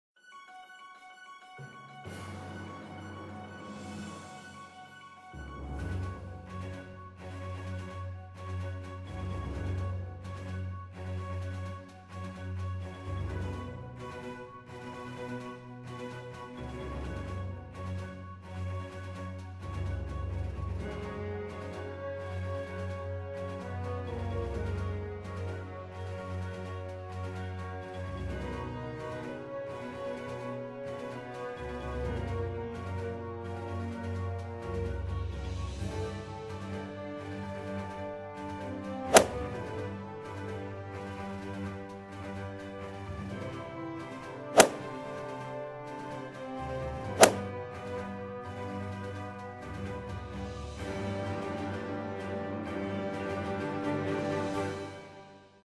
The Hanger Drill: For Consistently Pure Ball Striking
All you need is a plastic Hanger and a 7 or 8 Iron and you will be compressing the ball like a pro in no time Flat.

Place the hanger under your trail armpit, but do NOT keep your trail elbow connected to your body.  Be sure to make the bottom of the hanger as level to the ground as possible when you are in your golf stance.

Take the club back and hinge your wrist until you feel the hanger touch your lead arm and then lead the downswing by shifting and rotating your belt buckle towards the target.

Keep the lead arm touching the hanger until you are halfway back down (meaning lead arm parallel to ground) and then allow the club to release to and through impact.

Pure Ball Striking is just a plastic hanger away!!